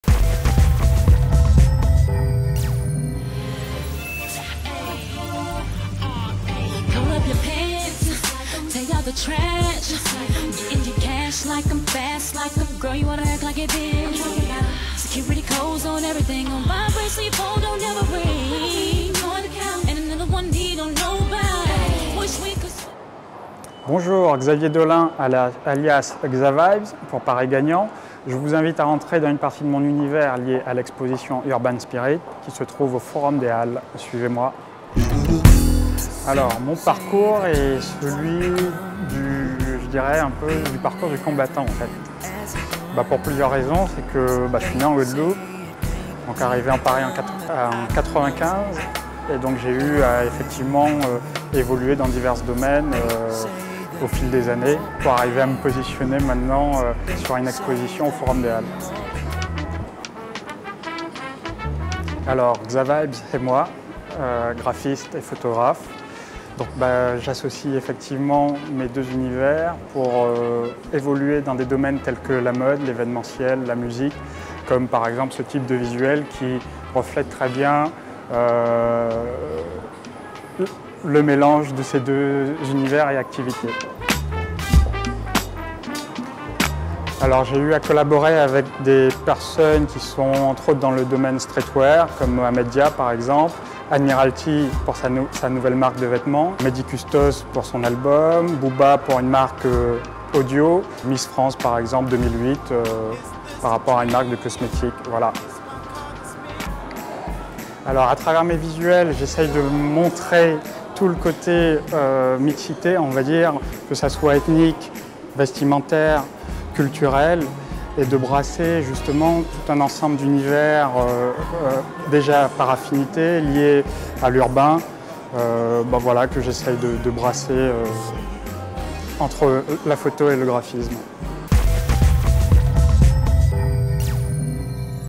Pari Gagnant - Xavier DOLLIN dit XAVIBES
Talentueux photographiste. Il mele le cliche au graphisme et le resultat est surprenant. La preuve avec cette exposition